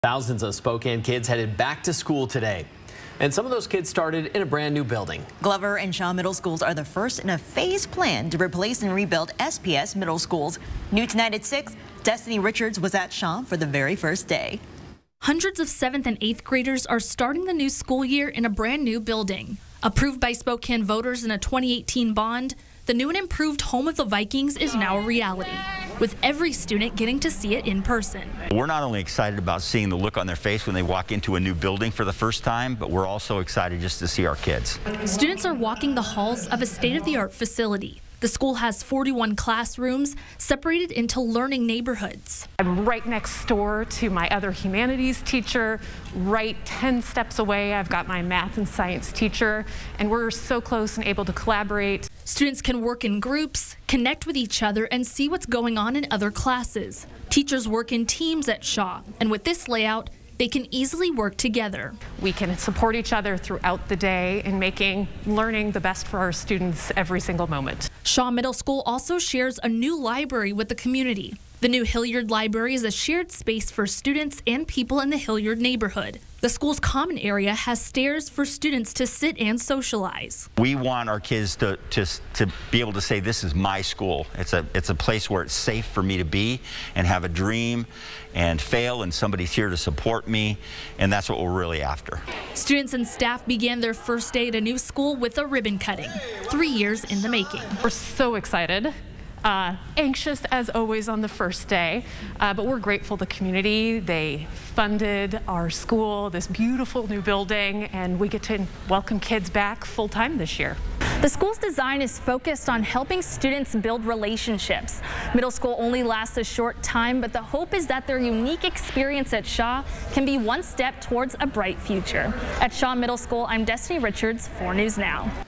Students in Shaw Middle School start school in new building
4 News Now's Destiny Richards reports